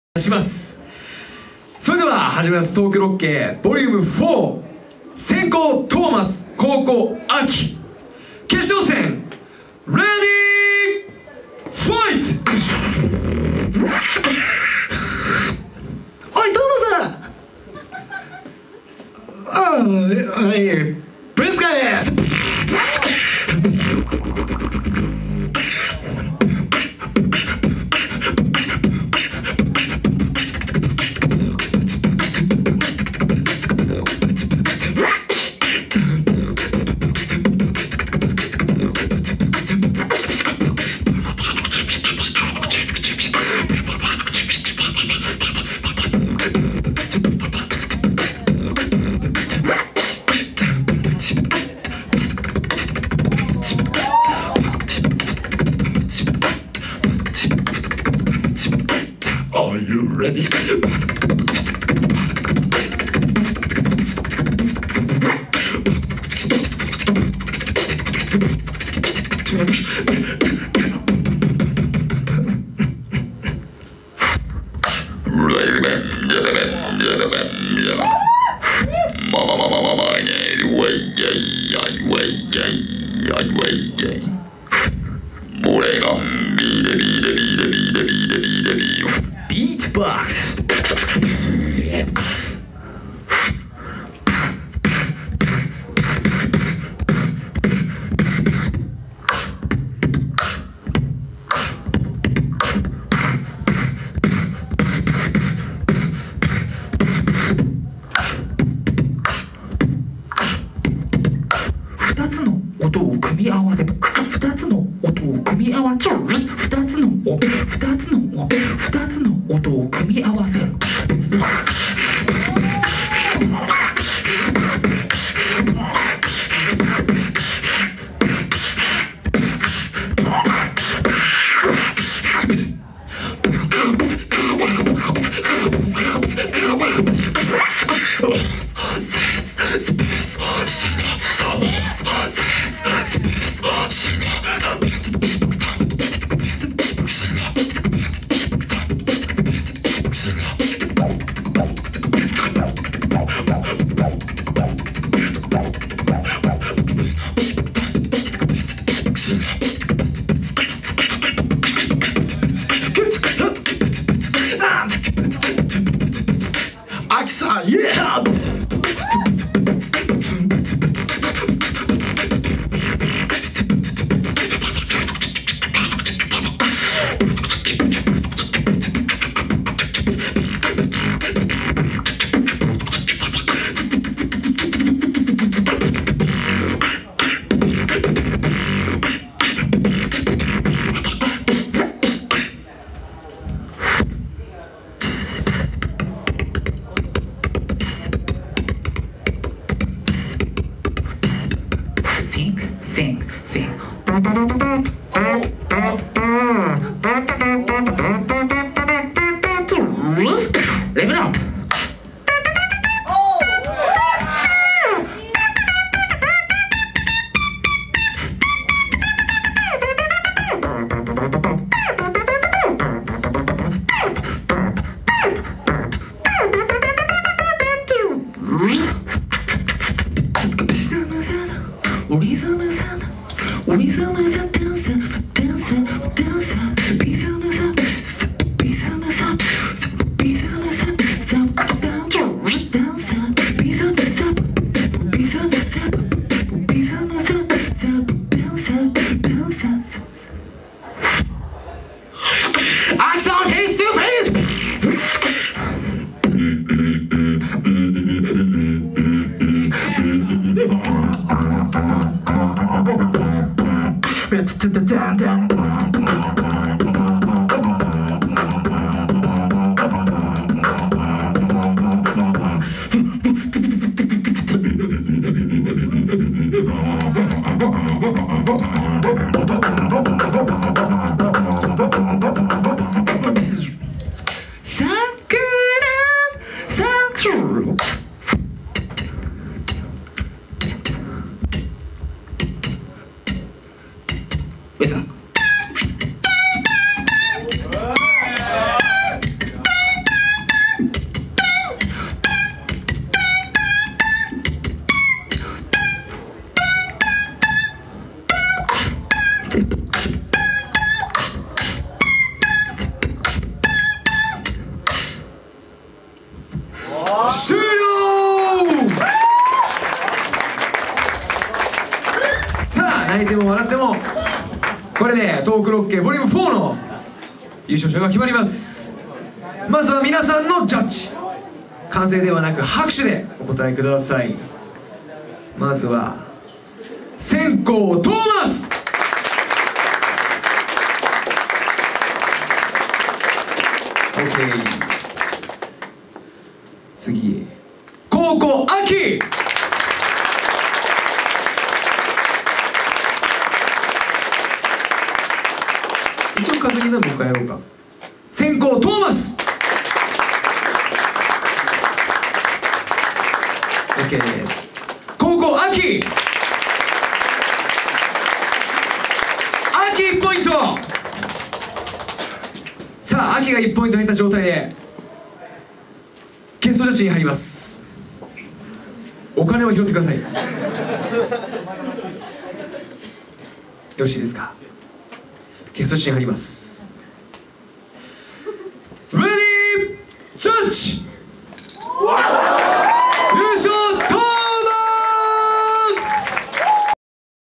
noisescape-東北六景-vol.4 決勝 Tomas(宮城) VS 秋(宮城)
BEATBOX BATTLE東北大会vol.4　2013 1/15@MACANA
JUDGE 太華 SHIN　MC 孝太郎